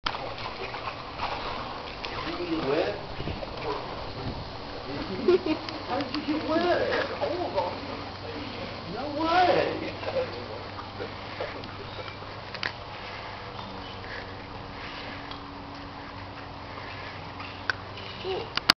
Turtle Rescue Appended
Finally got the vid to play. Short I know but I was covered in smelly, slimy mud.  Bad for digital cameras.